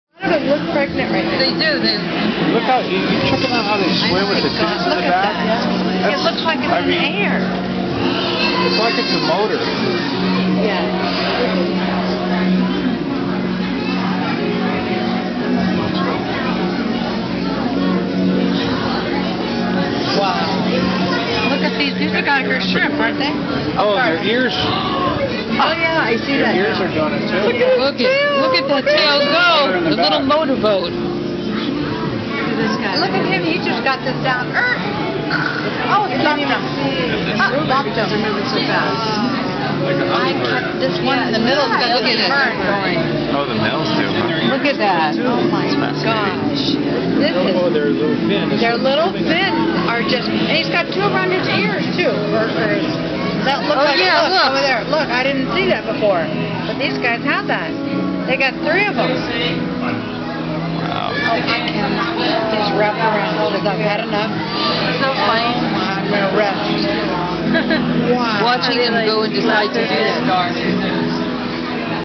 Sea Horses at the Monterey Bay Aquarium
These little wonders of creation are said to be guided by the Heart as noted by the protruding guidance of the chest. Majestic and magical role models for HUMANity.